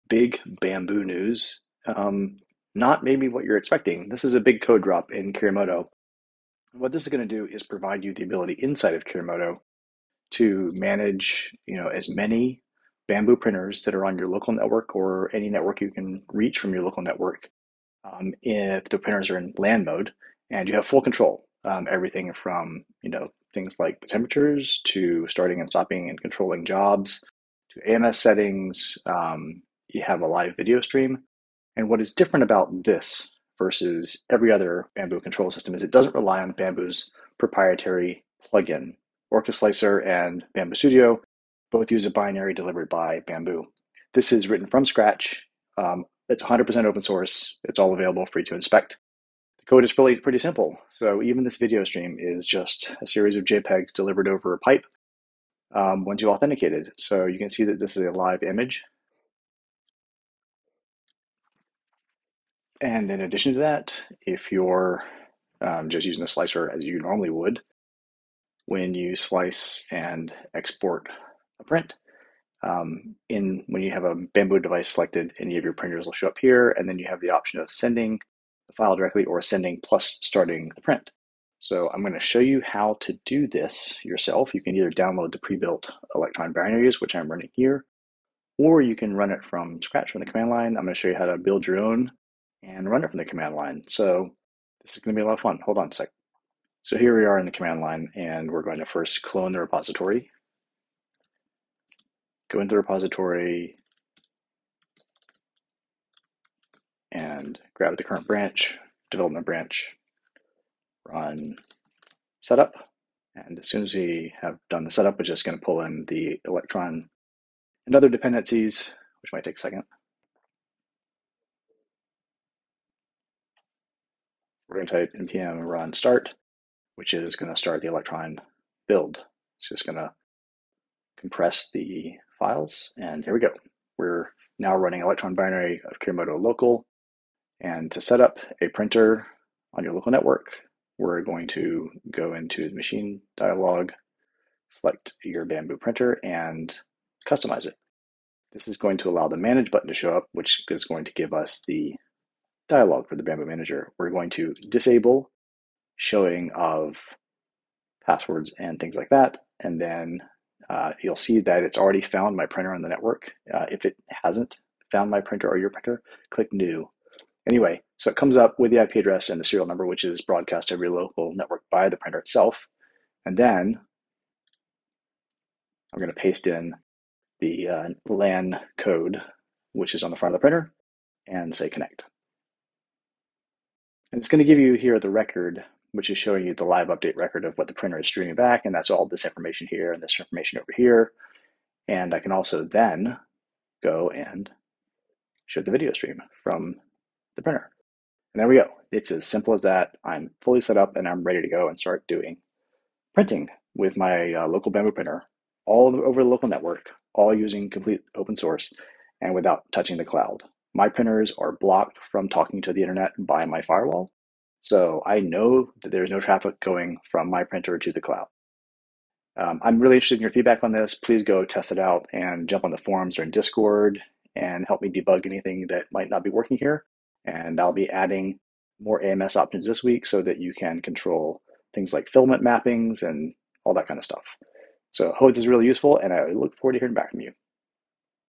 Free the Bambu!
New Kiri:Moto code drop that allows full control of your LAN mode Bambu printers that's entirely local and open source. Yes, I am hoping to get this into more mainstream slicers.

GridSpace and Kiri:Moto are going on 14 years of continuous development.